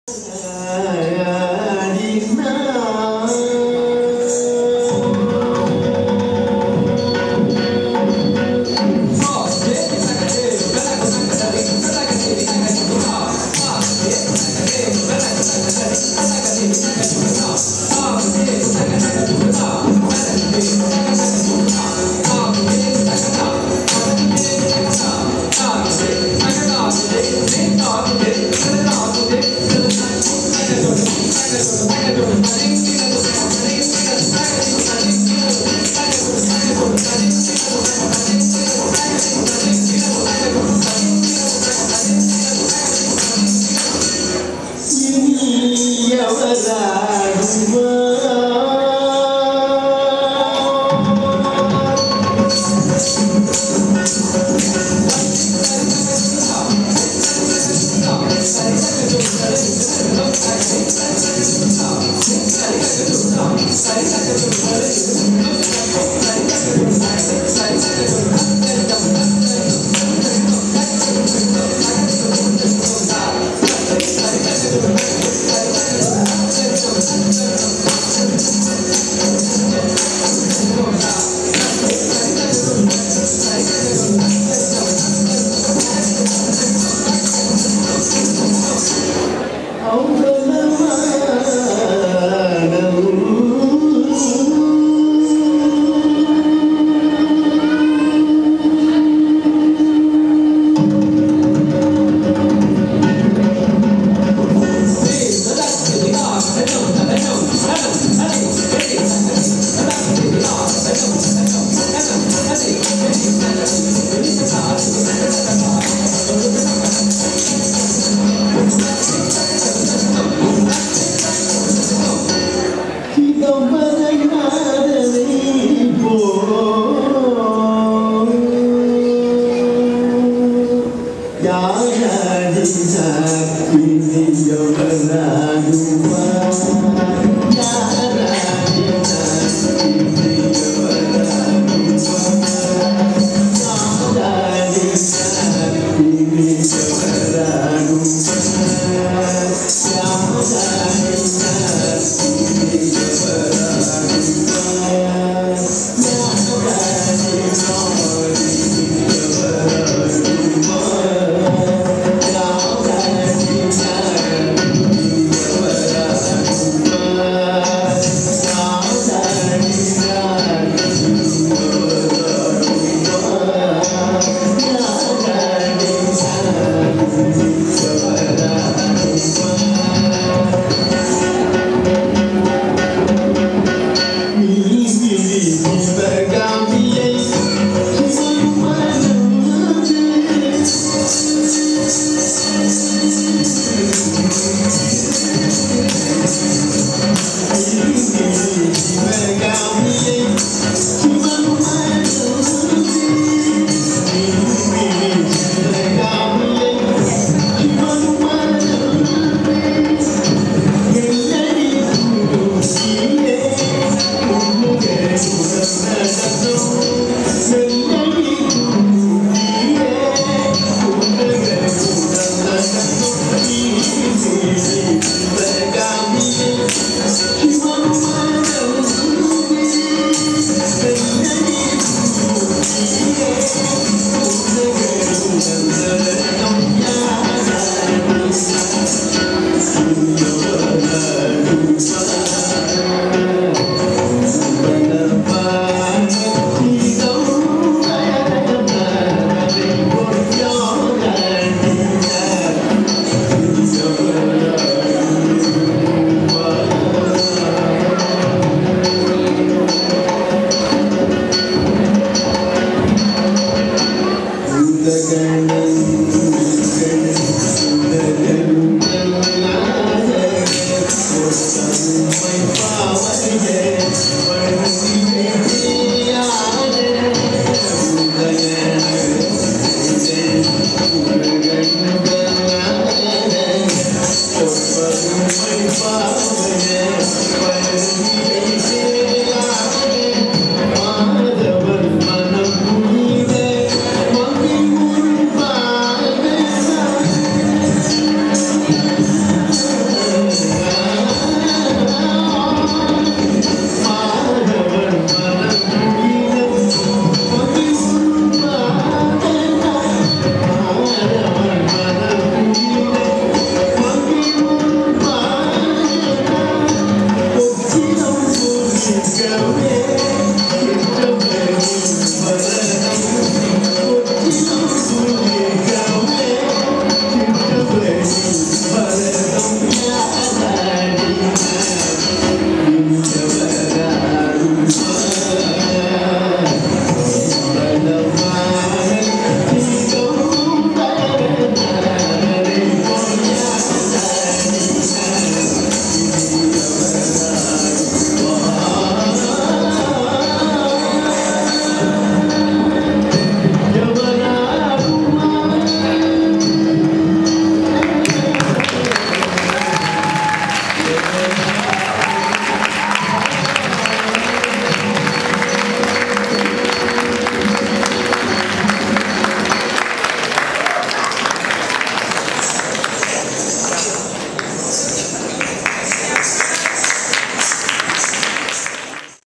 Vinutha and Namrata perform Bharath Natyam
Bharath Natyam at AIA 25th Anniversary Celebration